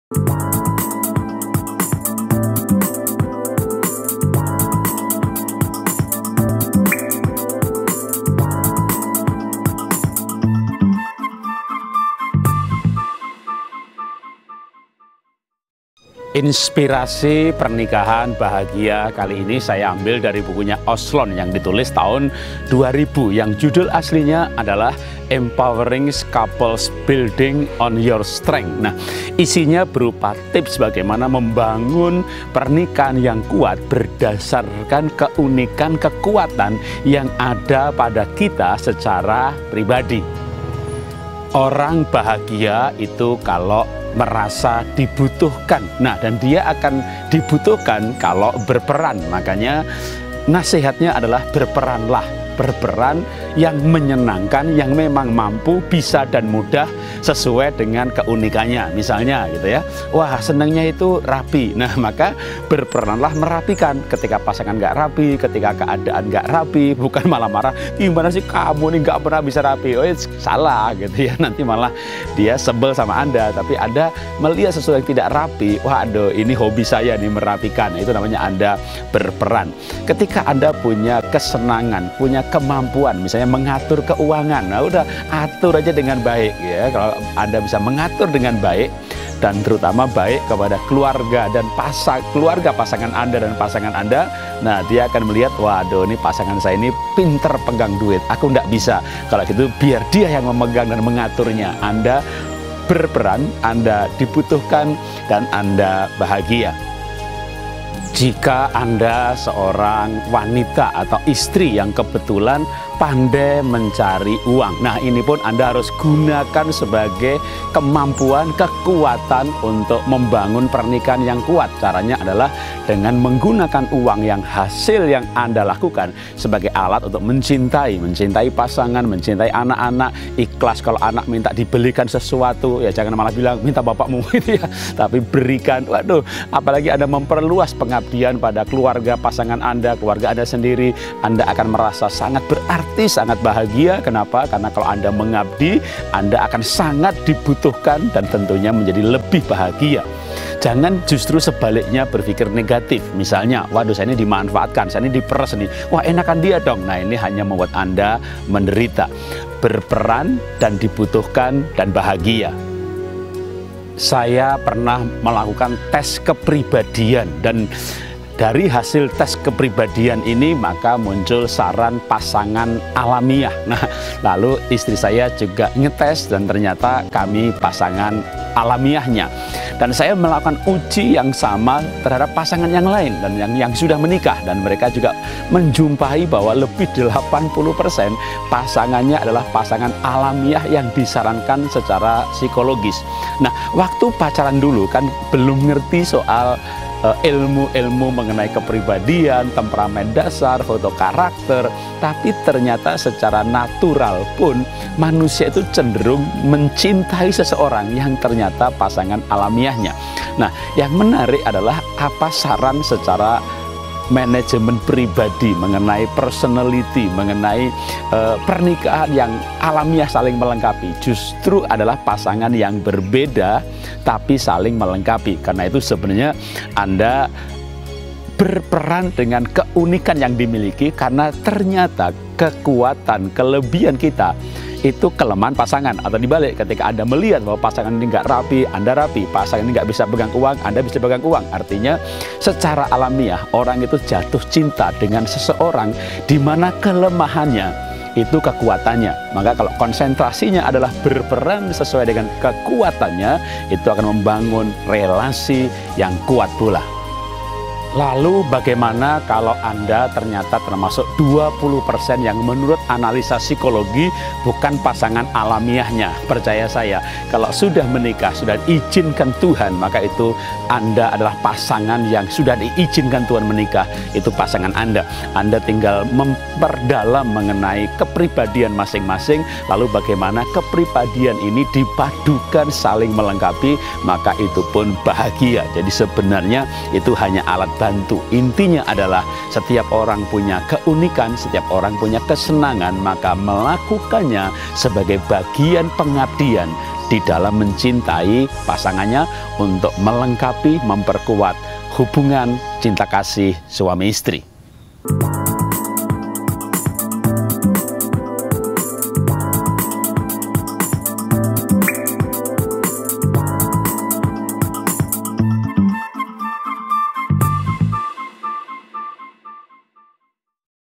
Berperan, dibutuhkan dan bahagia, Dr. Jarot Wijanarko M.Pd
Orang dihargai karena kontribusinya. Orang bahagia jika dia dibutuhkan dan akan dibutuhkan karena berperan. Karena itu daripada menunut pasangan berubah, lebih baik berperan sesuai keunikan, minat, bakat, kesenangan dan perhatiannya (passion) sehingga menjadi dibutuhkan oleh pasangannya.  Saling membutuhkan jauh lebih bahagia daripada saling menuntut berubah.
Dr Jarot Wijanarko M.Pd.